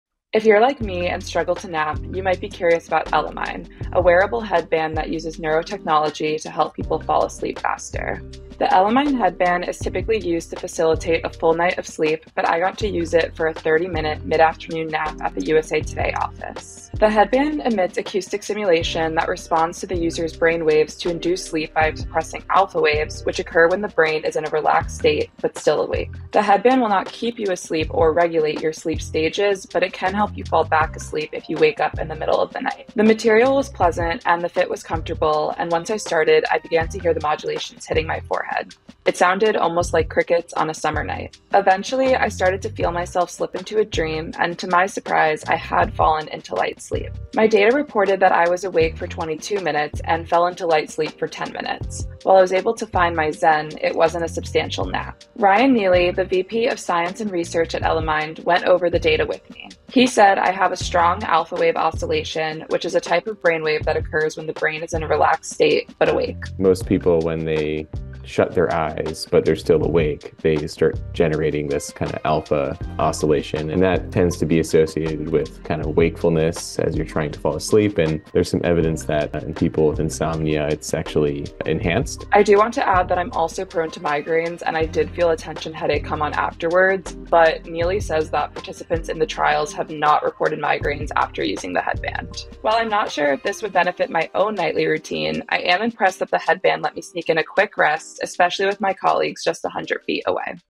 The Elemind headband aims to improve sleep quality with AI technology
USA TODAY wellness reporter Alyssa Goldberg tested the Elemind headband, which aims to help you fall asleep faster by utilizing sleep science and AI.